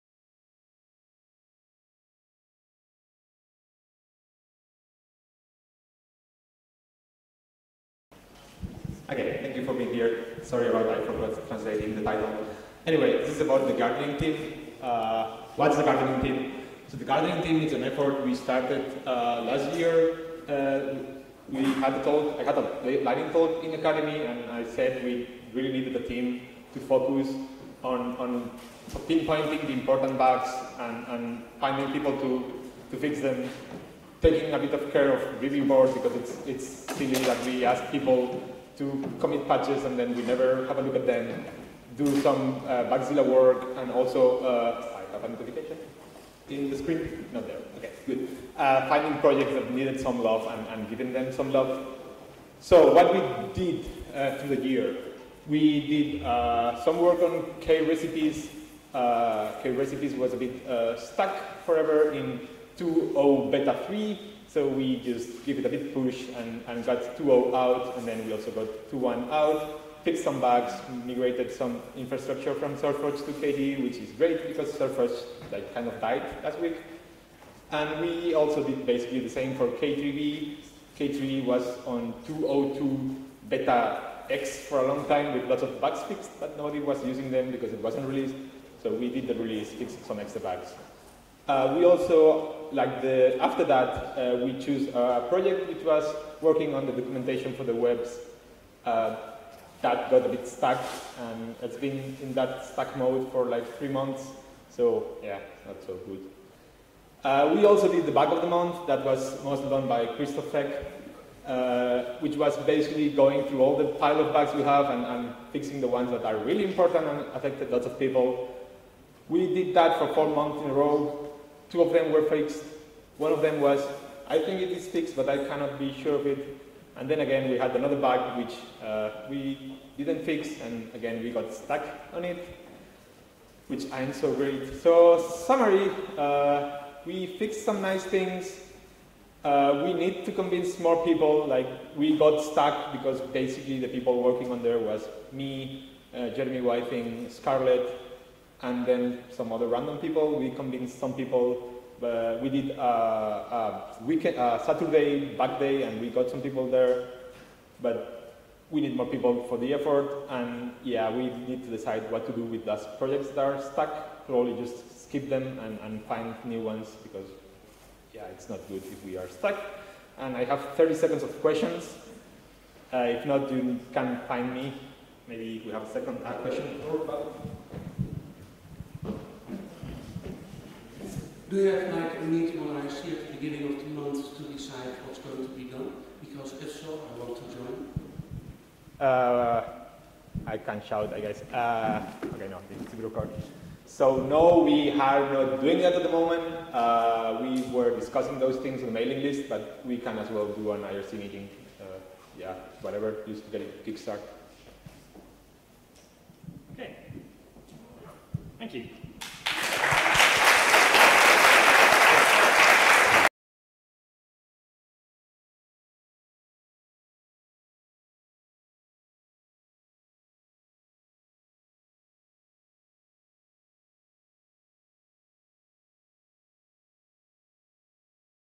Albert Astals Cid - One Year of Gardening - Akademy 2015
A quick update of what the gardening team has achieved in its less than one year of existence and the challenges ahead.

The gardening team helped new releases of k3b, krecipes, found and helped people fix important bugs, we can all do more if you come to the talk and decide to join us ;)